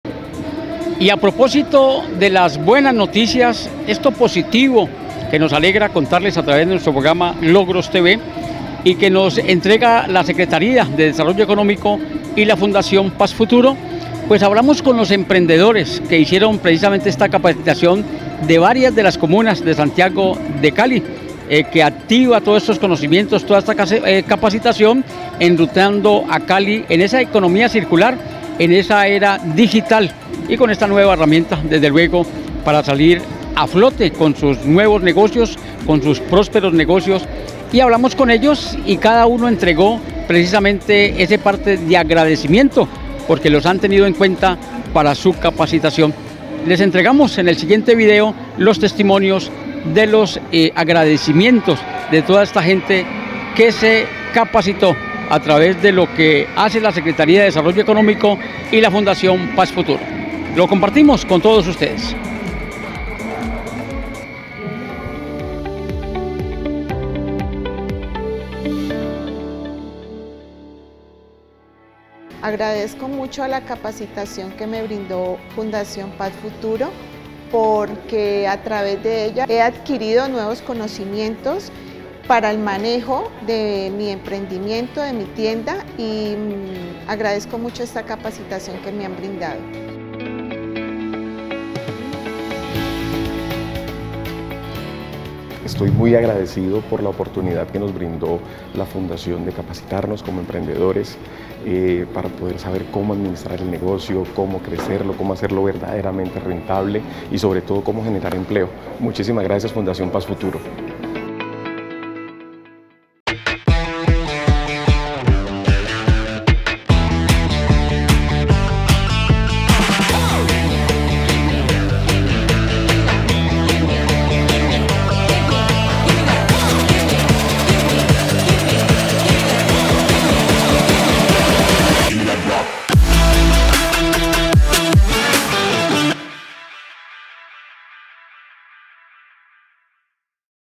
Fundación Paz Futuro - Programas formativos - Beneficiarios de este proyecto
Beneficiarios de este proyecto, que enruta a Cali en la Economía Circular, comparten su mensaje de agradecimiento tras haberse aventurado en esta experiencia formativa donde cientos de emprendedores caleños, certificaron aprendizajes con los que llevarán al éxito sus negocios aprovechando la era digital.